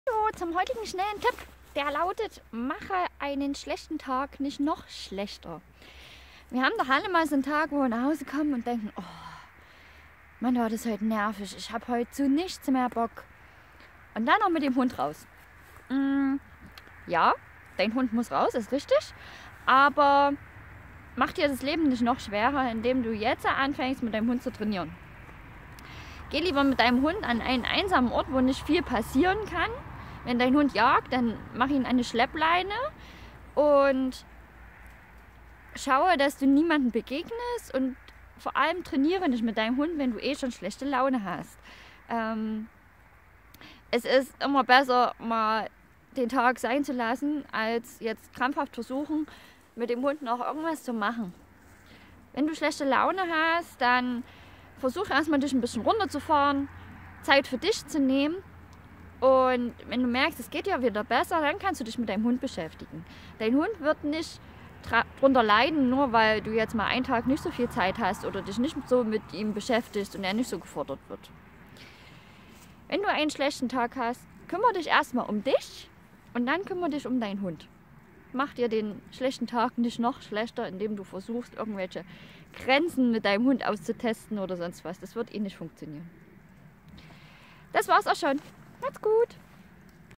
Mach dir einen schlechten Tag nicht noch schlechter!
T2G Quick Tipp

Hast du schlechte Laune, dann trainiere nicht mit deinem Hund, sondern gehe an einem Ort spazieren an dem du niemanden triffst.